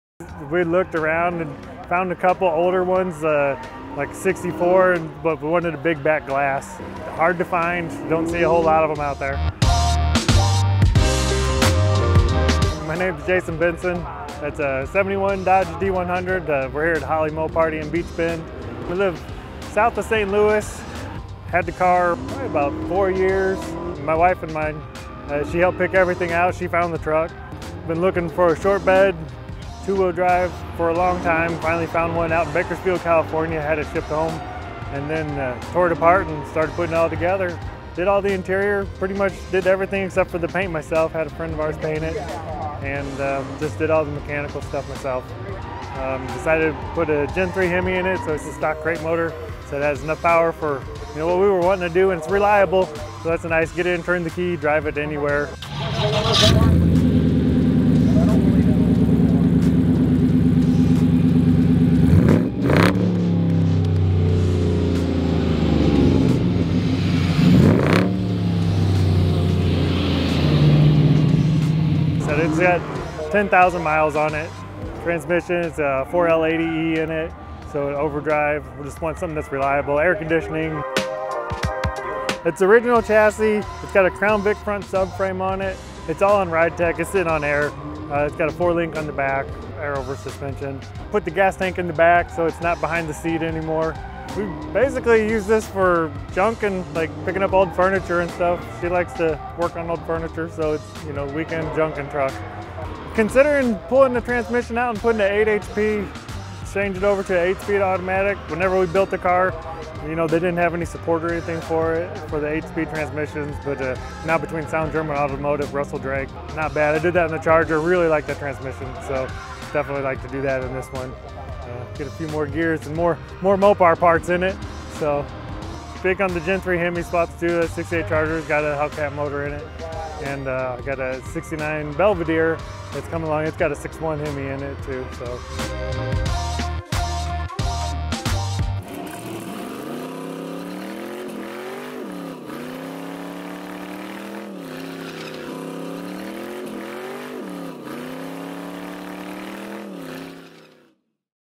Holley MoParty 2021: This 1971 Dodge D100 Rocks Modern Gen 3 Hemi Power and the Perfect Stance
Jason Benson might have had the cleanest 1971 Dodge D100 at Holley MoParty 2021. The truck is a full restomod build featuring Gen 3 Hemi power, and the stance is courtesy of air-ride suspension. Jason did all the mechanical work himself and the cruiser has already racked up 10,000 road miles on the new drivetrain.